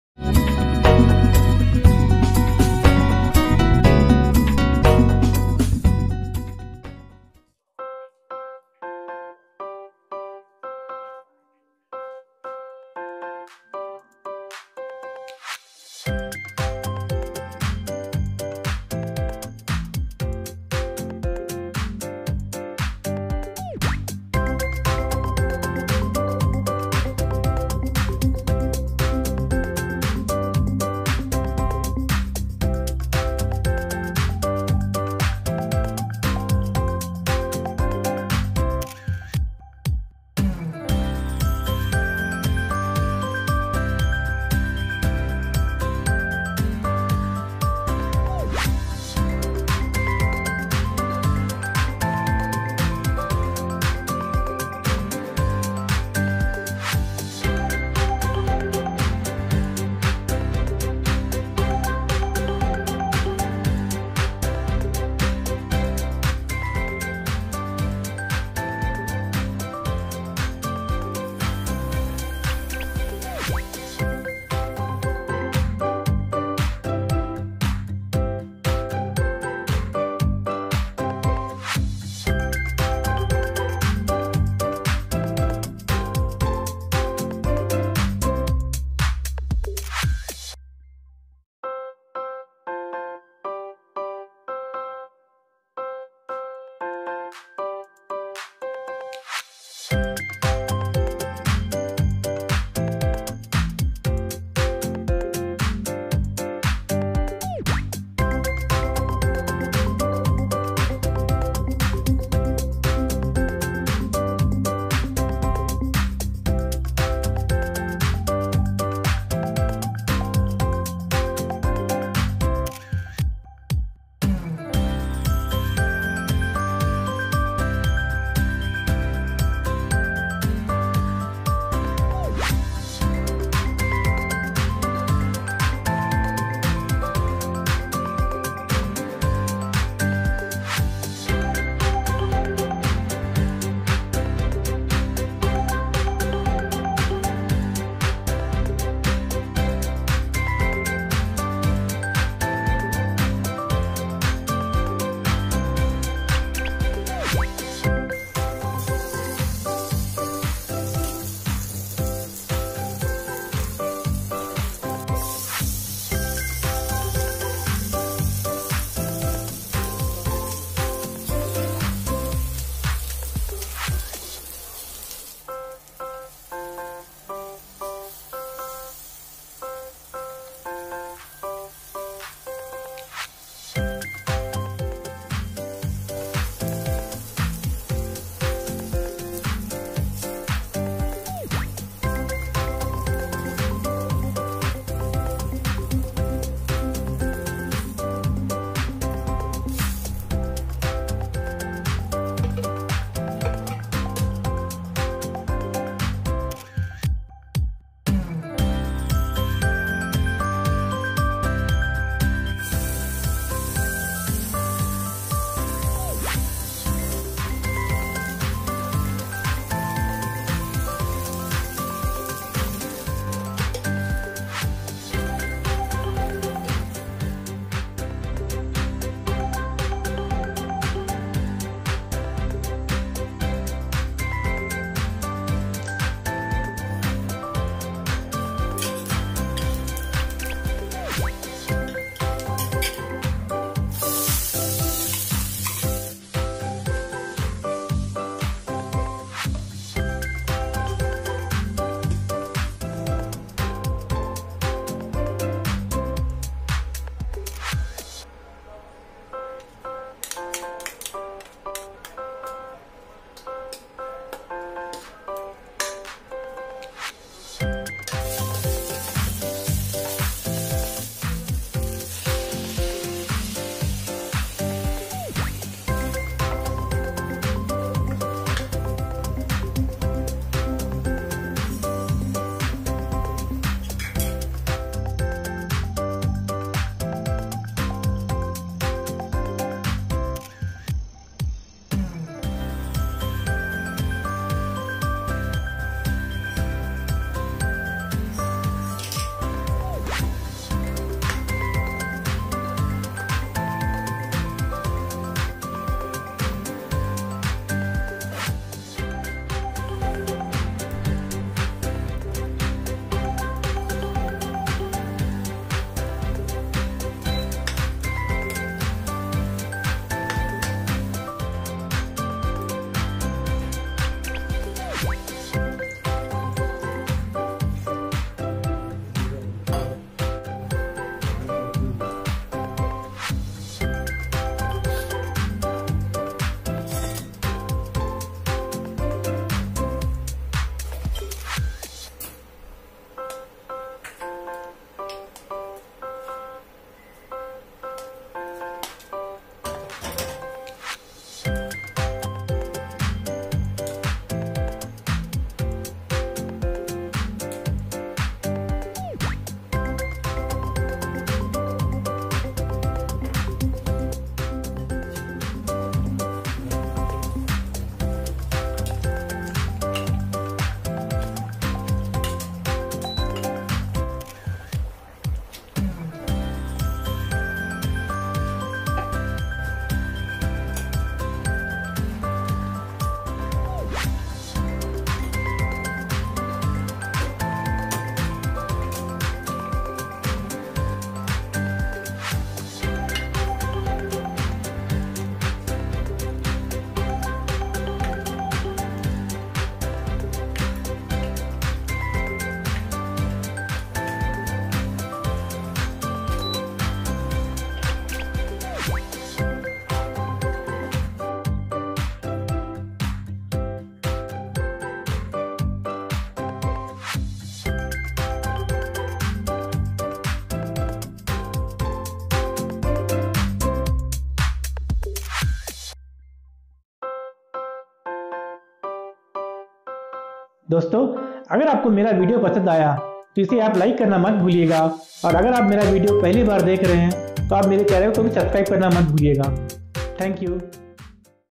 Paneer Tikka Masala | पनीर टिक्का मसाला | How To Make Paneer Tikka Masala | Paneer Recipe
Learn how to make Paneer Tikka Masala at home in less oil. Paneer Tikka Masala is a vegetarian version of world-famous chicken tikka masala. Traditionally paneer is first marinated and then grilled in a tandoori oven or clay oven which imparts a great smoky flavor. Since we don't have such tandoori oven at home, we have to grill the paneer on a grilling pan or Tawa.